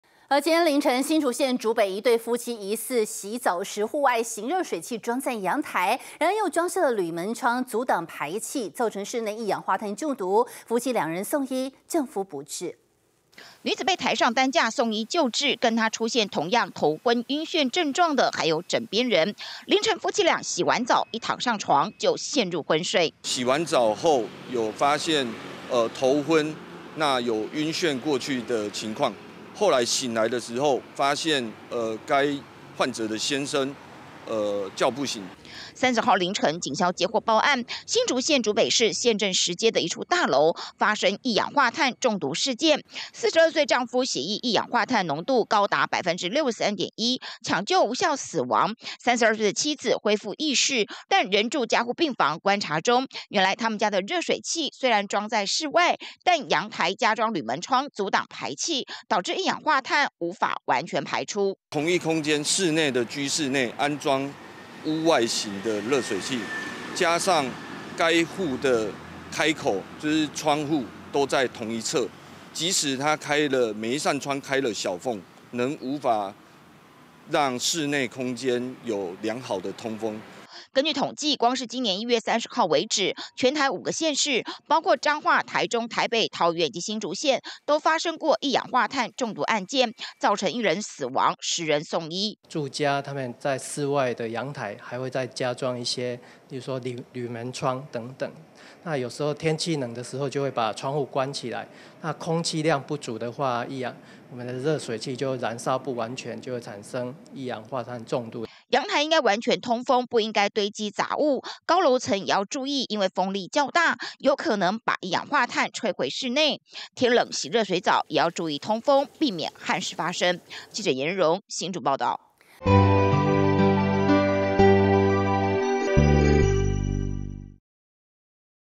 慟！ 熱水器裝陽台卻「緊閉鋁窗」 夫妻一氧化碳中毒「釀1死」
今（30）日凌晨新竹縣竹北發生一起一氧化碳中毒意外，一對夫妻疑似天冷洗澡時，雖然戶外型熱水器裝在陽台，但又裝設鋁門窗阻擋排氣，造成室內一氧化碳中毒，夫妻兩人送醫，丈夫不治。而光是113年1月還沒過完，全台就發生5件一氧化碳中毒，造成1人死亡、10人送醫。